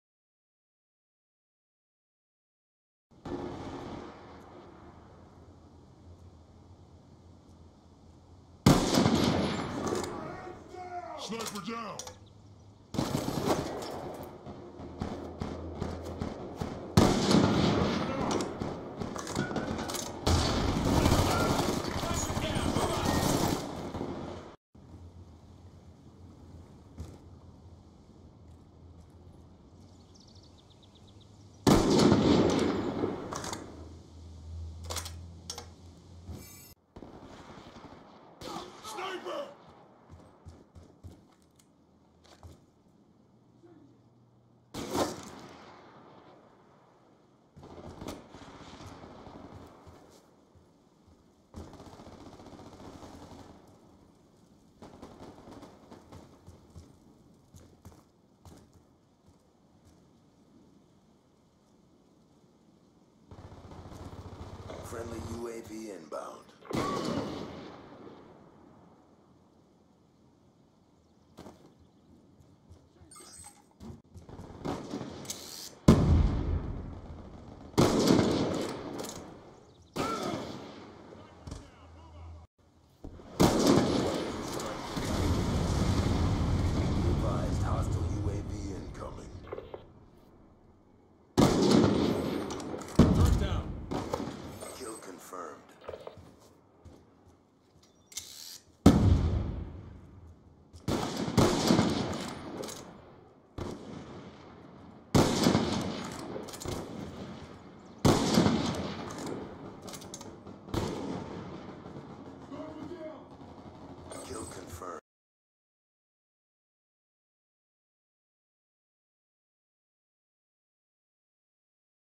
Iron site Bolt-action fun
I've been wanting to play against this guy (YourMaker) but we are usually on the same team. He's pretty good with the sniper rifle and I like a challenge. In the end he for sure got me more than I got him but I did get him a few times. These are my favorite moments.
(and one "wtf how did that miss?" moment)